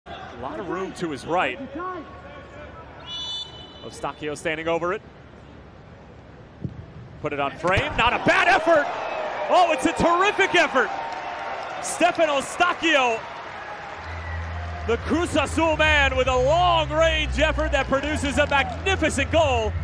Goal of the Day presented by Toyota
What a free kick! 🎯⚽

Stephen Eustáquio 🇨🇦 scores the Goal of the Day presented by @ToyotaLatino